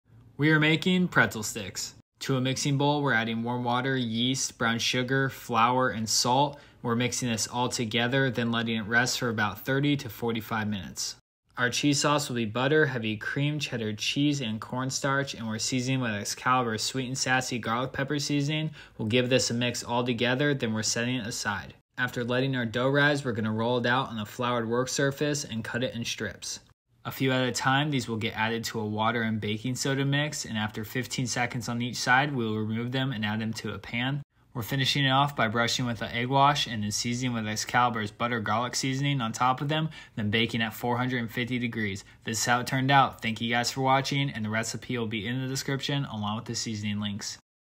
Homemade Pretzel Sticks
Pretzel Sticks

Pretzels
1 cup of warm water
1 packet of yeast
1/4 cup of sugar
2.5 cups of flour
Salt
Mix and let rest for 45 minutes
Cut into strips
Add to a water baking soda mix that is simmering
Let both sides stay in for 15 seconds then remove and pay dry
Brush with egg wash and season with Sweet and Sassy Garlic Pepper
Bake at 450 until golden brown

Cheese Sauce:
1 tbsp of butter
1/2 cup heavy cream
4 oz of cheddar cheese
1/2 tbsp of corn starch